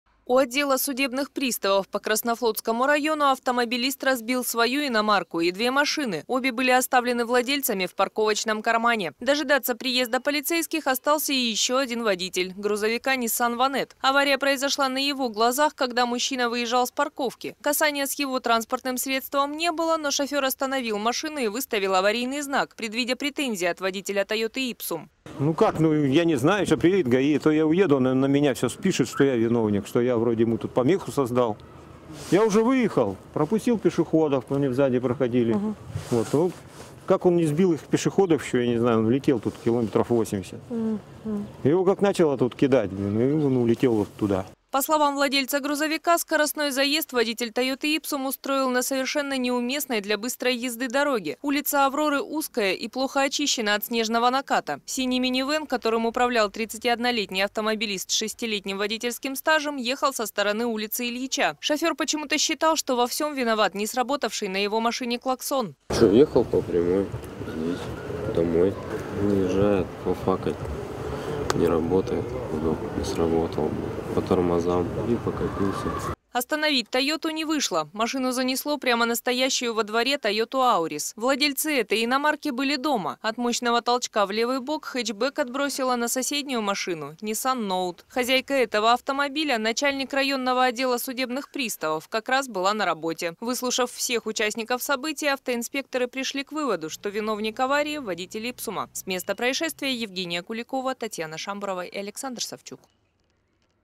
Разогнался и разбил 3 машины.MestoproTV
У отдела судебных приставов по Краснофлотскому району автомобилист разбил свою иномарку и две машины - обе были оставлены владельцами в парковочном кармане.